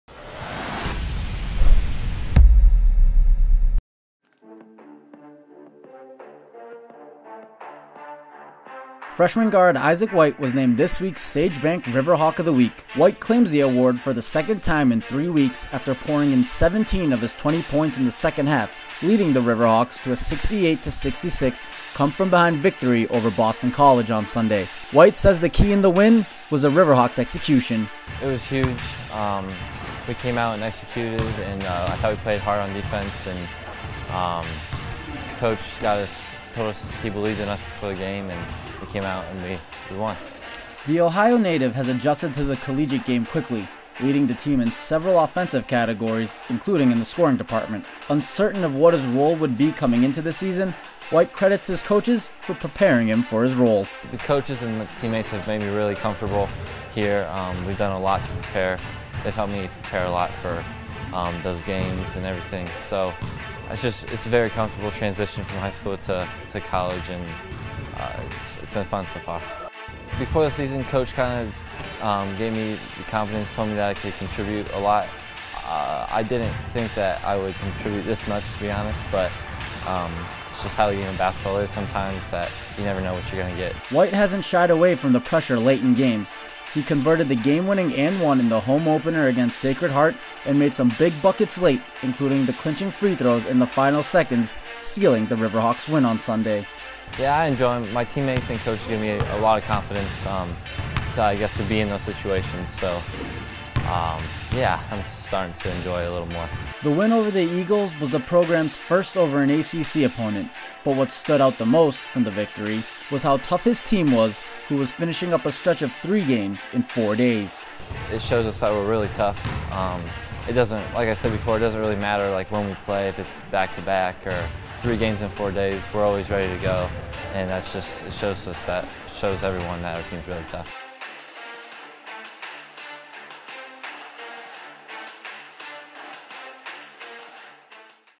Isaac White: River Hawk of the Week
Isaac White was named River Hawk of the Week after scoring 17 of his game-high 20 points after the break and leading his team to a 68-66 comeback victory over Boston College.